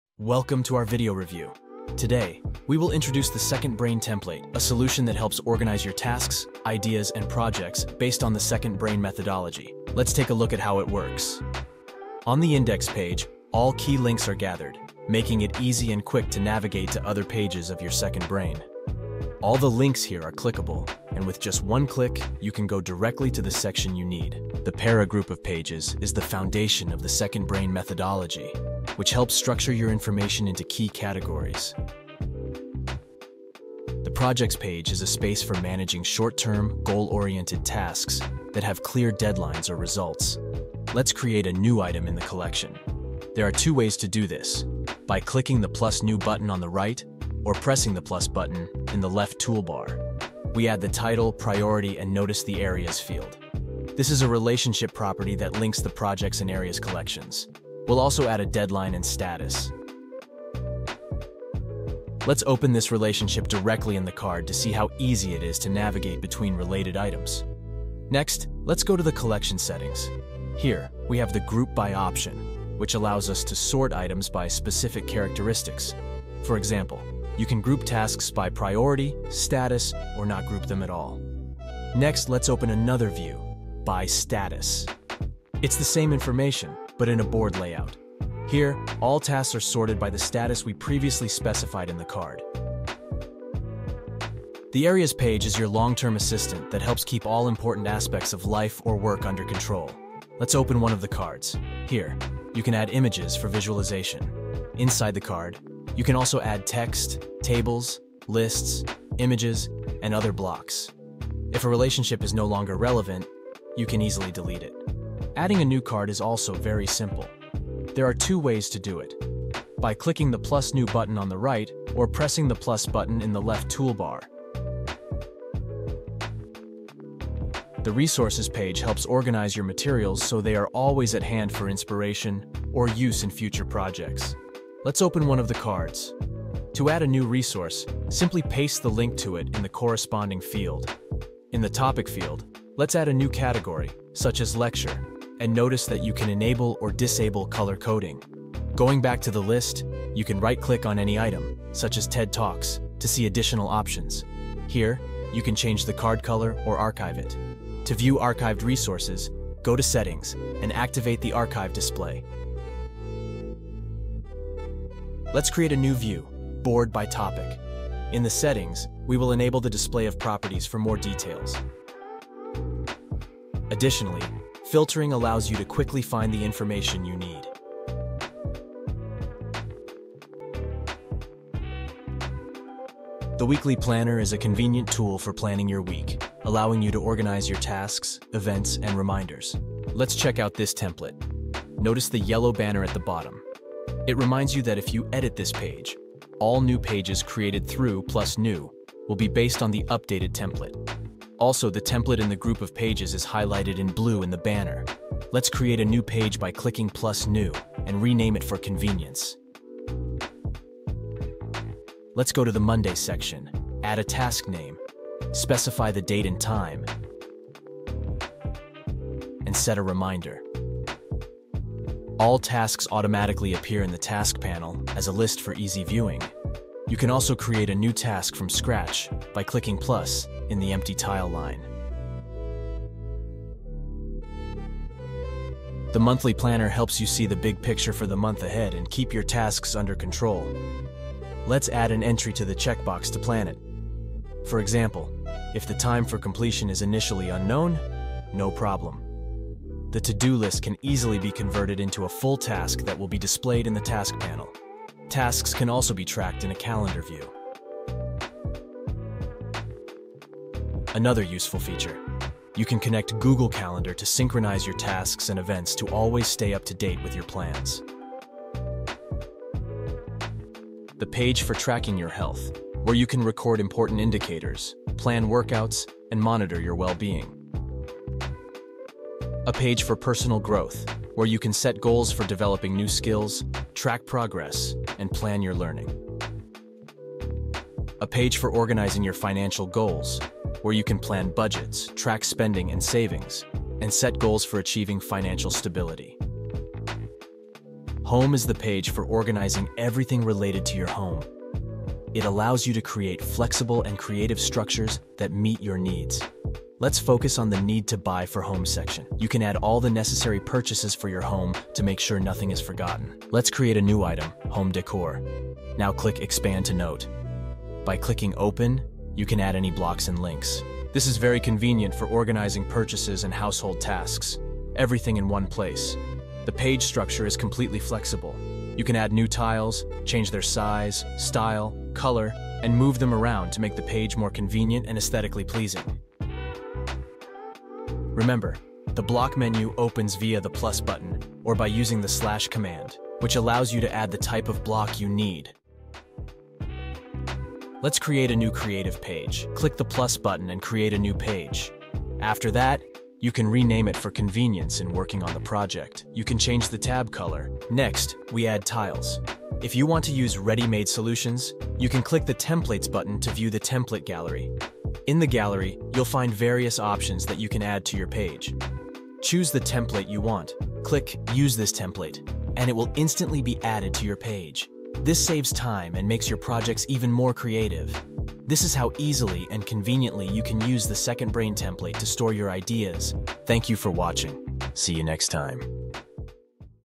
Second Brain: Organize Your Projects, Ideas, and Tasks Easily!
🧠 Say goodbye to scattered notes and chaotic to-do lists! 🧠
In this video, we’ll show you how to set up and use the Second Brain template to organize your work and life like never before.

Need to jump to a specific part? Here are the timestamps:
0:00 - Intro
0:12 - Index Page
0:33 - Projects
1:33 - Areas
2:06 - Resources
3:01 - Weekly Planner
3:54 - Monthly Planner
4:29 - My Health
4:38 - Self-Development
4:46 - Financial Goals
4:56 - Home
5:46 - Creative Page
6:18 - Outro

KEEP IN TOUCH!